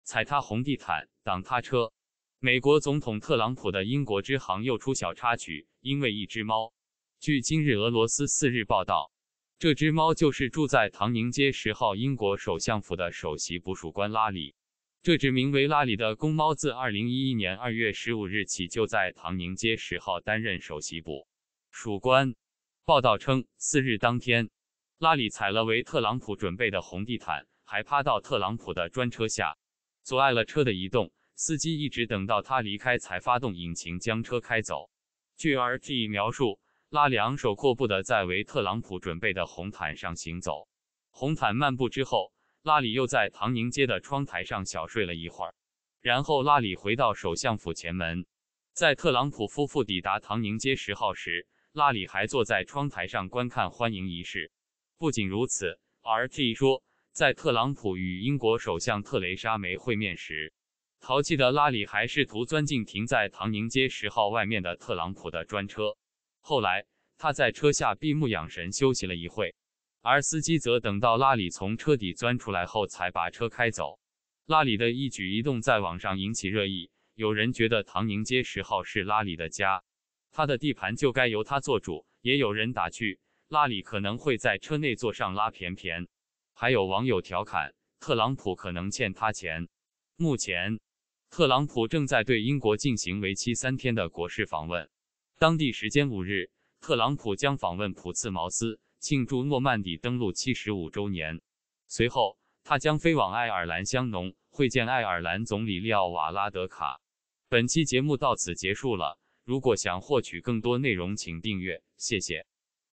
特朗普访英，喵出爪了！
踩他红地毯，挡他车……

美国总统特朗普的“英国之行”又出小插曲，因为一只猫。

据“今日俄罗斯”4日报道，这只猫就是住在唐宁街10号英国首相府的“首席捕鼠官”拉里。这只名为拉里的公猫自20……